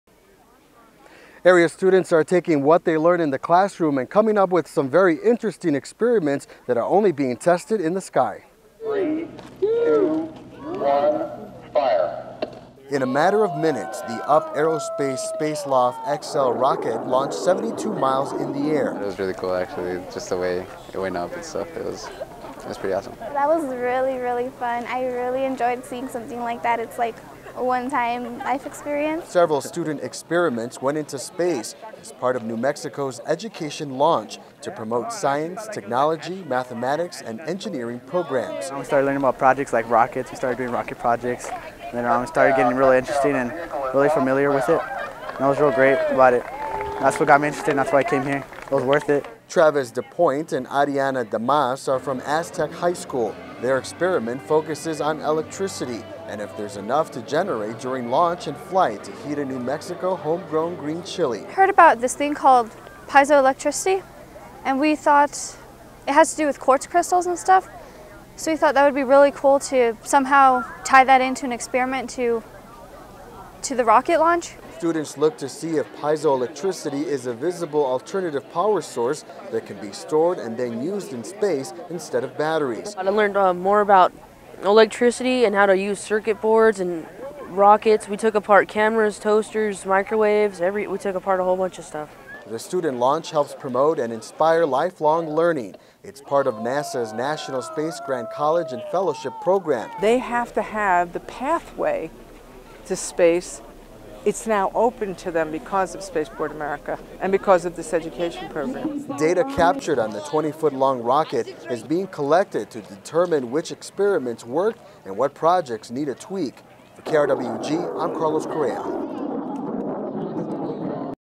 Student Launch
05.24.11 (UPHAM, NM) -- Students in New Mexico, Texas and Arizona recently participated in an out of this world experience, bringing months of work and preparation to a close.

In a matter of minutes the UP Aerospace Spaceloft XL Rocket launched 72 miles in the air.

"It was really cool actually, just the way it went up and stuff, its pretty awesome," said Josue Acosta, student at Chaparral High School.

"That was really, really fun, I really enjoyed something like that its like a one time life experience," said Desiree Perez, student at Chaparral High School.

Several student experiments went into space as part of New Mexico's education launch to promote science, technology, mathematics, and engineering programs.  Johnny torres is a 9th grader at chaparral high school.

"We just started learning about rockets and doing rocket projects and I started getting really interested, really familiar with it and it was really great.  That's what got me interested and that's why I came here," said Johnny Torres, 9th grader at Chaparral High School.

Travis Duponte and Ariana Dimas are from Aztec High School.  Their experiment focuses on electricity and if there's enough to generate during launch and flight to heat a New Mexico home grown green chili.

"We heard about this thing called piezoelectricity and we thought it has to do with crystals so, we thought it would be really cool to some how tie that into an experiment to the rocket launch and then we're from New Mexico so green Chile is a major part of the state so, we thought that would be cool to get some how use the electricity to get a stove, a little oven, and roast a piece of chilly," said Dimas.

Students look to see if piezoelectricity is a visible alternative power source that can be stored and then used in space instead of solar arrays or batteries.

"I learned more about electricity and how to use circuit boards, rockets, we took apart cameras, toasters, microwaves, we took apart all bunch of stuff," said Duponte.

The student launch helps promote and inspire lifelong learning.  It's part of NASA's National Space Grant College and Fellowship Program.

"They have to have the path way to space, it's now open to them because of Spaceport America and this education program," said Pat Hines, director of New Mexico Space Grant Consortium.

Data captured on the twenty-foot long rocket is being collected to determine which experiments worked and what projects need a tweak.

Reported by Carlos Correa